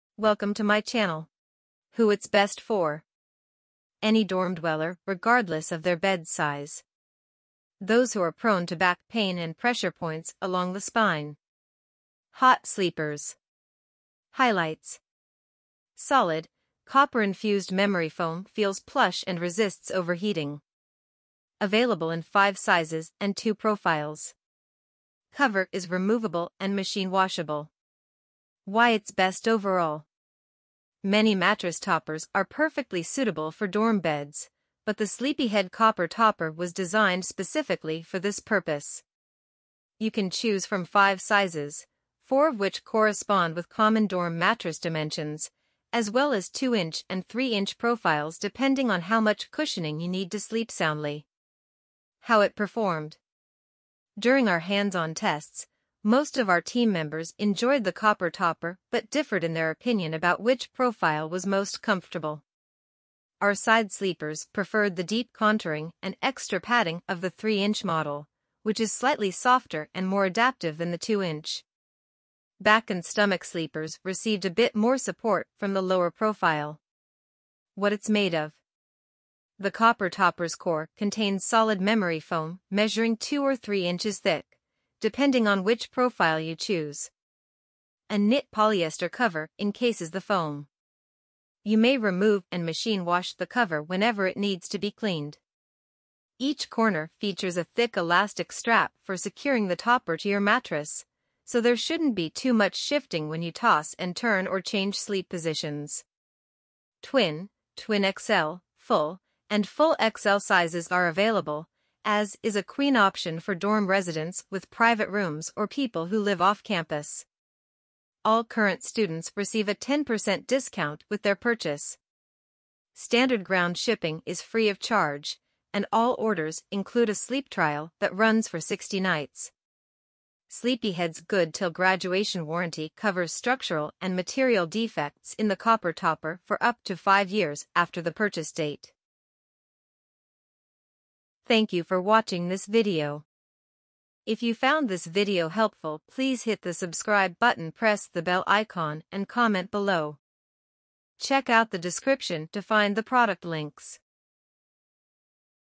Sleepyhead Copper Topper Review in 2023
in this video we will show CSleepyhead Copper Topper Review.

in this Sleepyhead Copper Topper Review, I cover everything from the beautiful appearance to the functionally of the item.